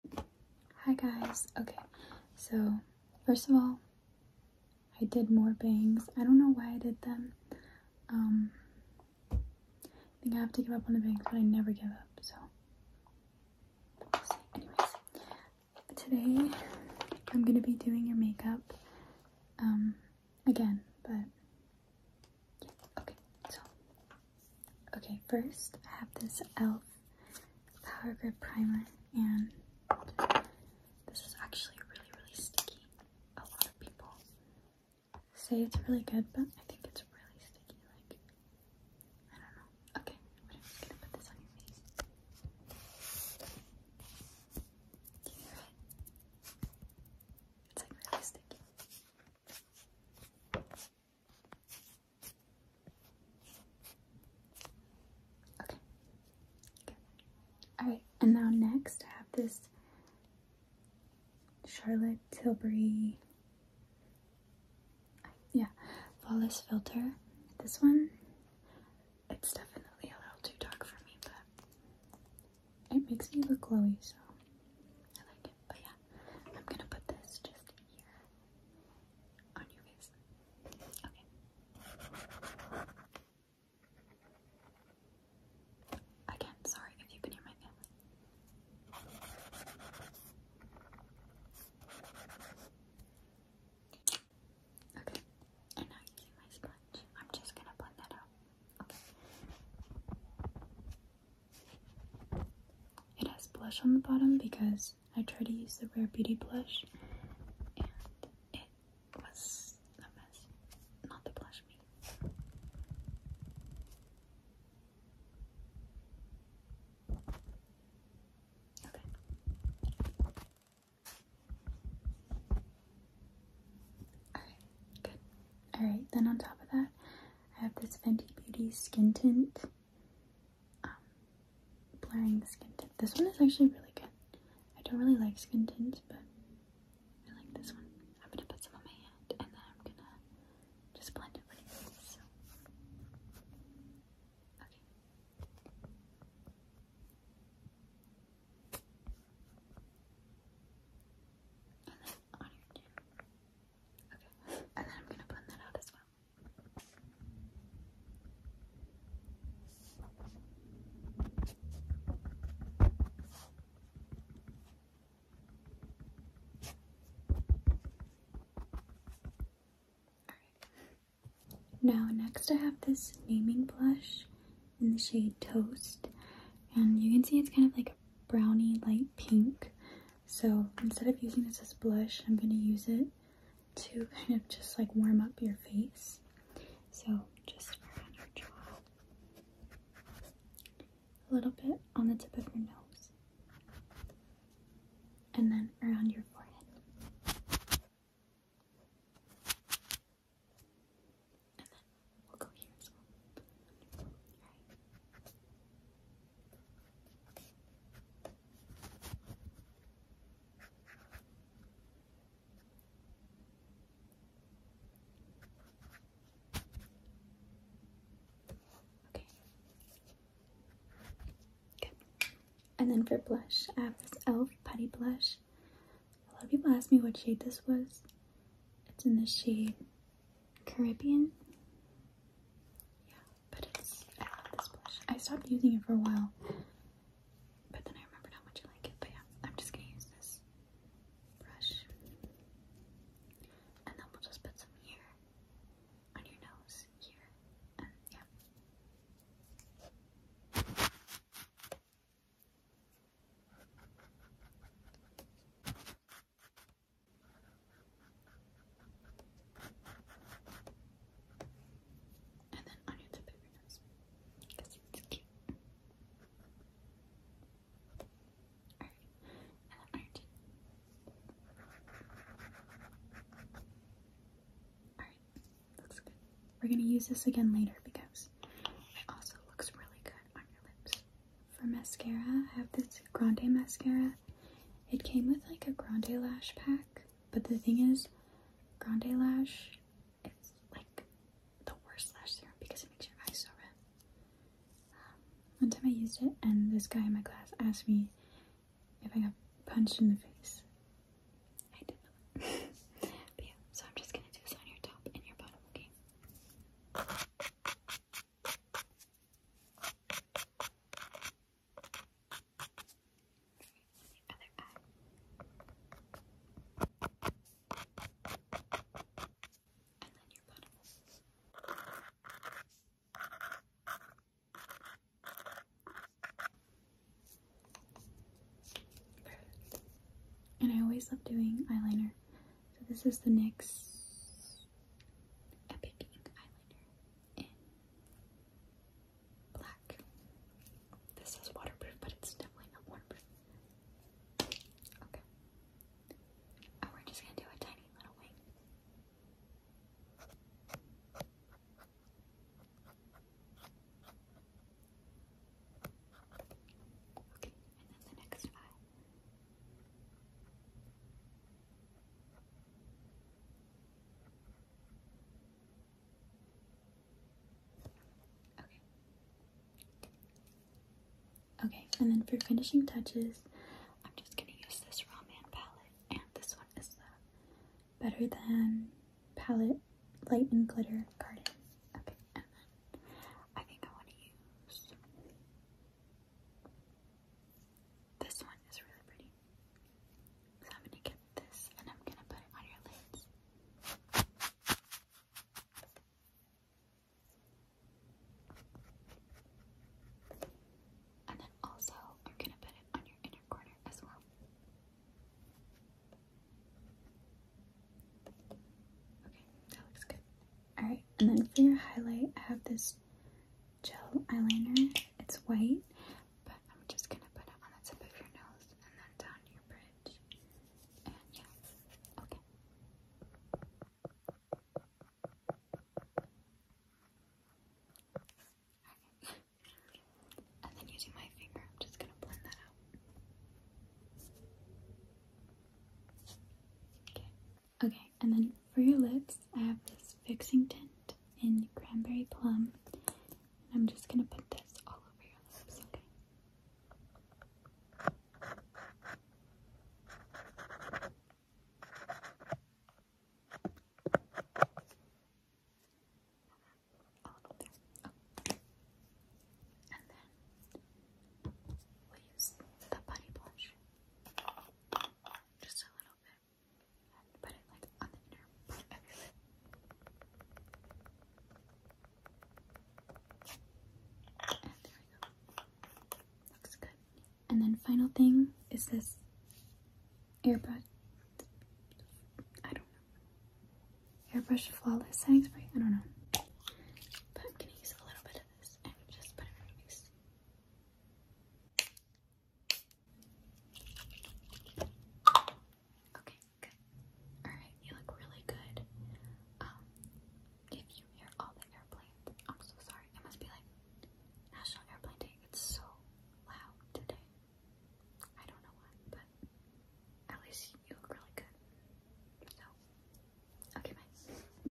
asmr 8 minute makeup
hellooo thank you for 55k :D it means a lot!! also sorry for the lack of videos lately, I just have had some weird weeks and zero motivation but I am back to normal (hopefully) and sorry for the abundance of airplanes in this video. everyone must be taking a flight today or something.

I also love doing makeup videos sorry if you don't like them..

yesstyle code: CAELNI2